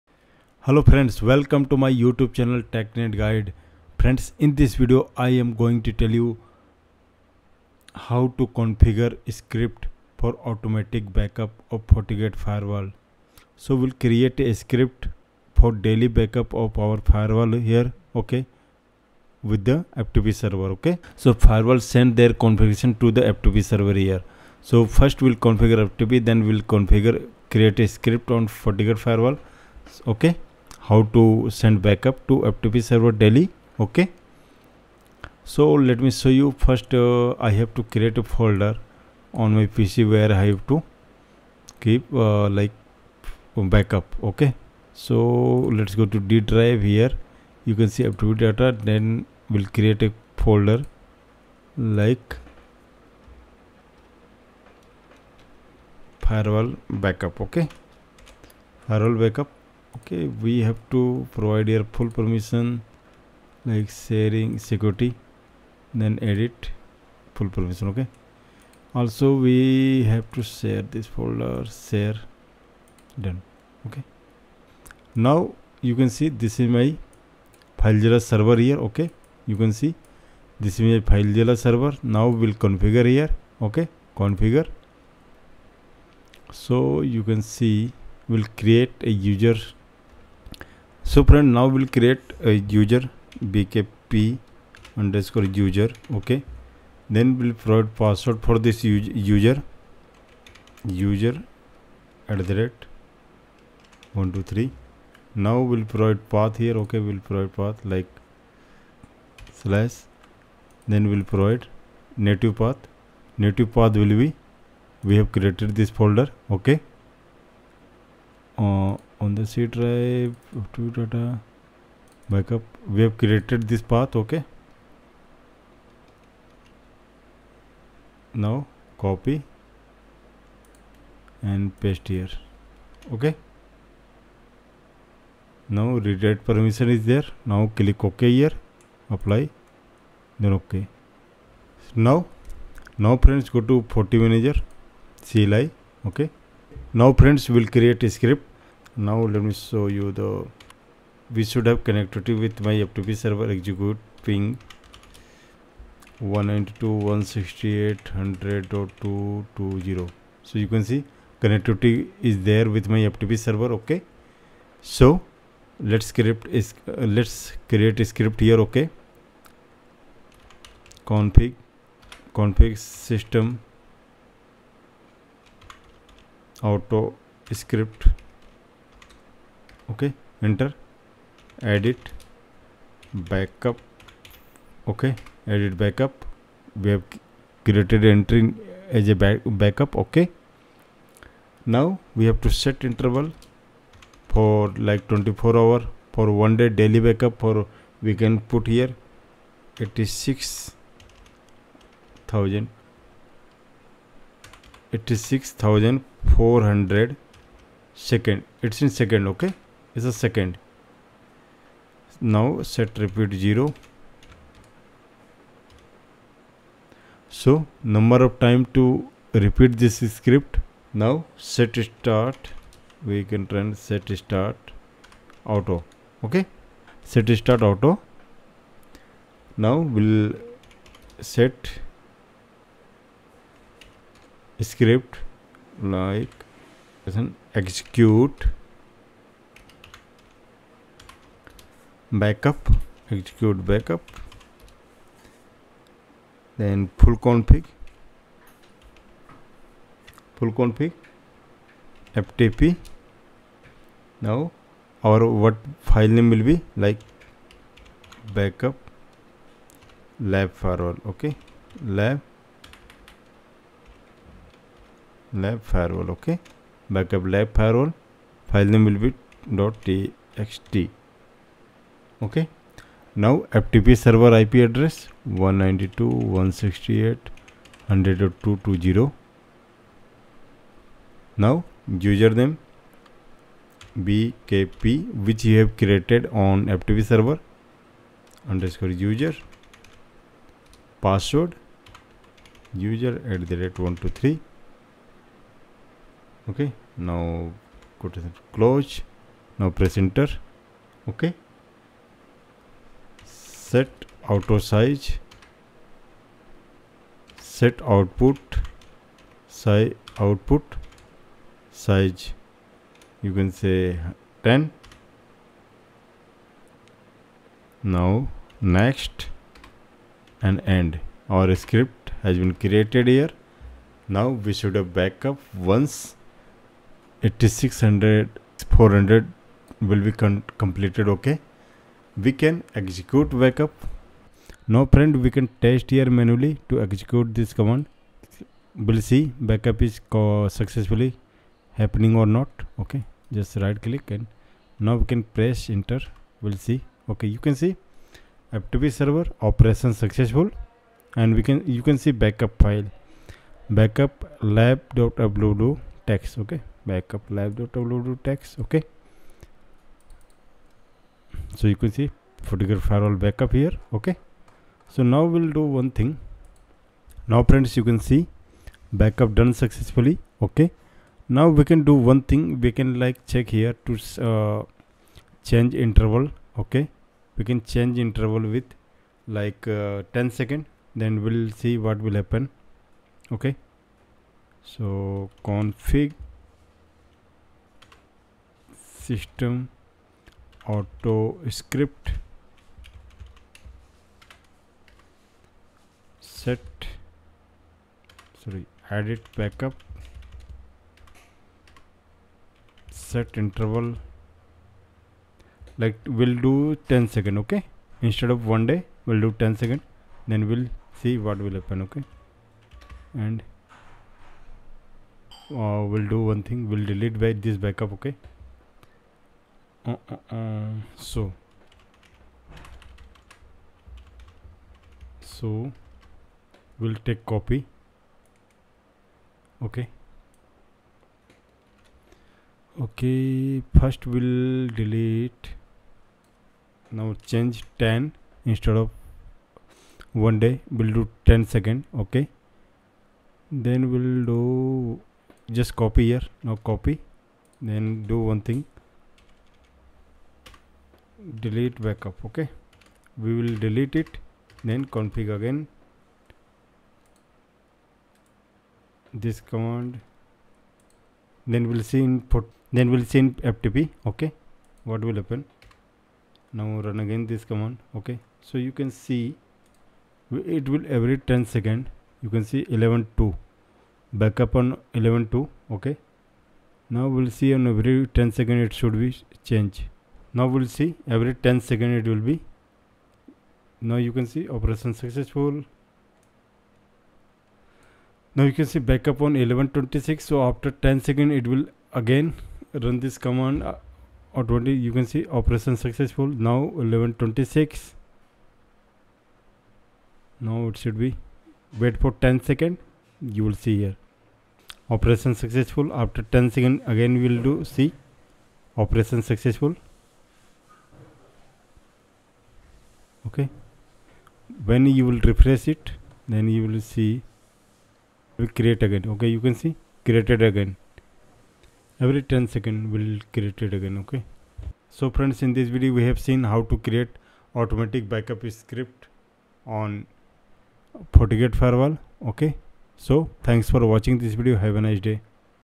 Create Fortigate Script for Automated Daily Configuration Backup to FTP Server
In this video, You will learn how to Create Script for Fortigate Automated Daily Configuration Backup to FTP Server filezilla
Configuring an automated script for daily FortiGate
How do I automatically backup my FortiGate Configuration?
How to create a script in FortiGate Firewall?
FortiGate automatic backup TFTP